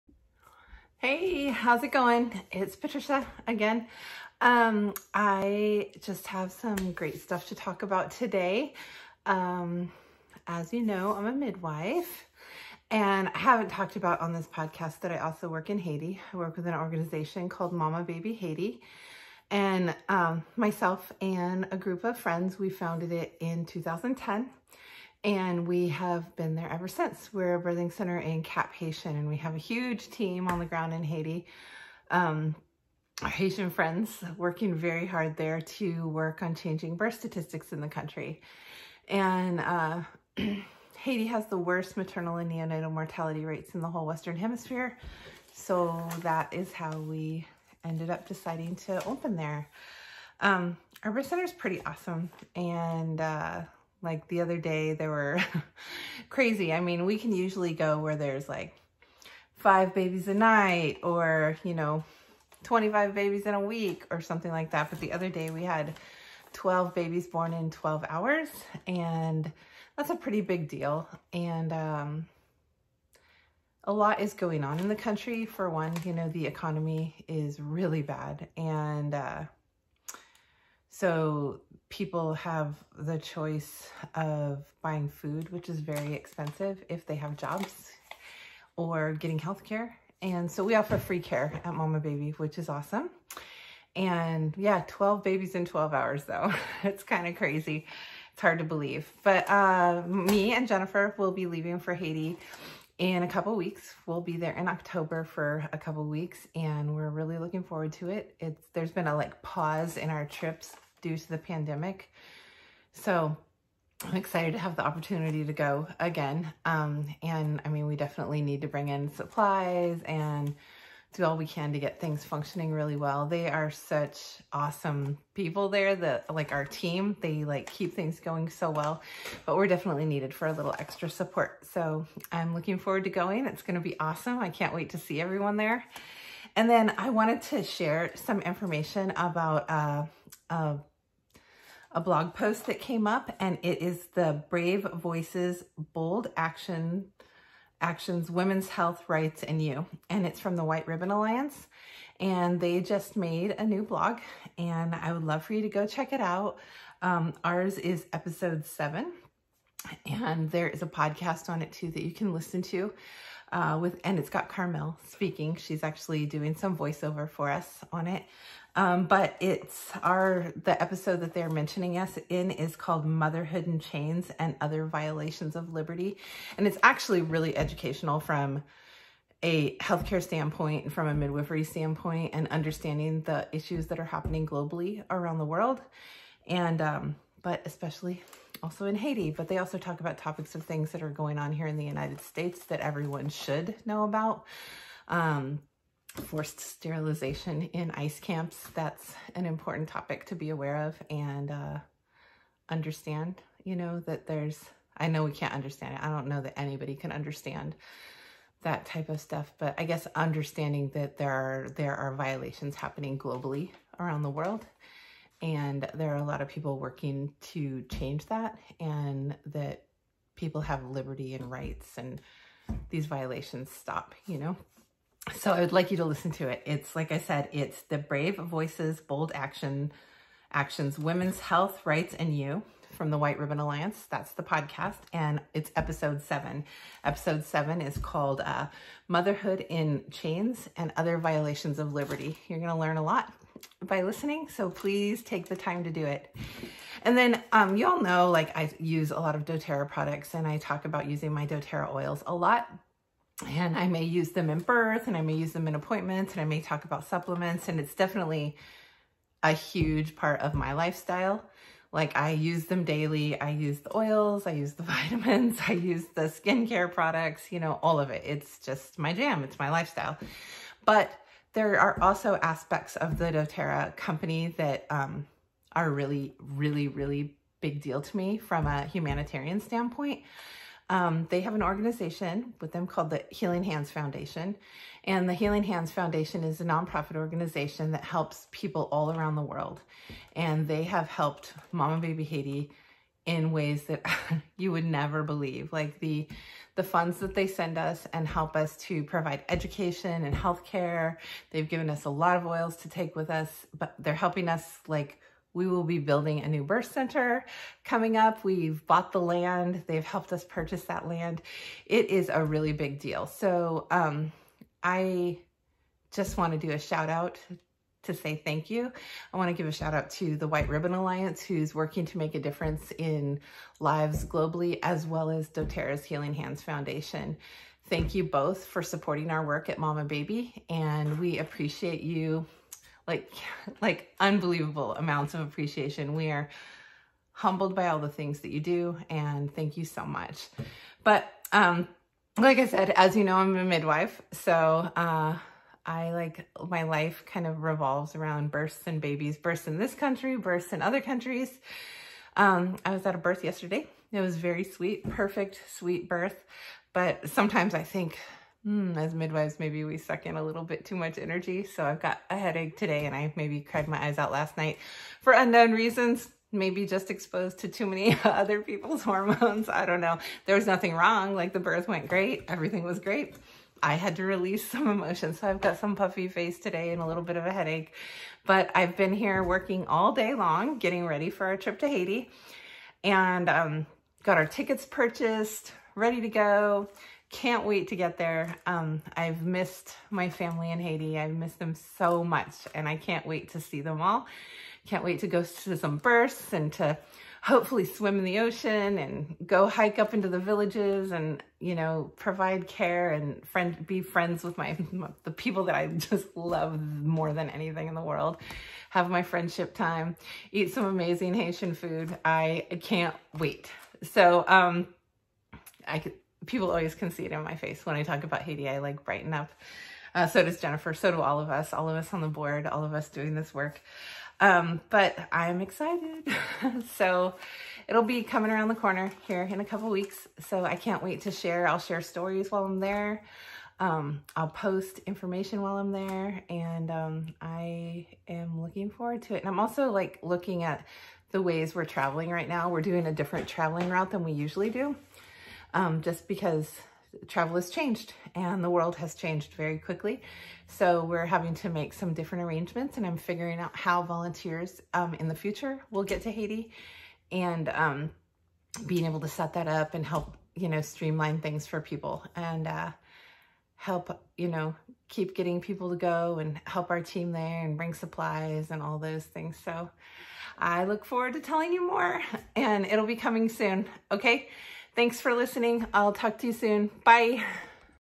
MamaBaby Haiti, the White Ribbon Alliance and the dōTERRA Healing Hands Foundation
I have a few things to say about MamaBaby Haiti
the White Ribbon Alliance blog and podcast-Brave Voices Bold Actions  Women’s Health, Rights and You
and the dōTERRA Healing Hands Foundation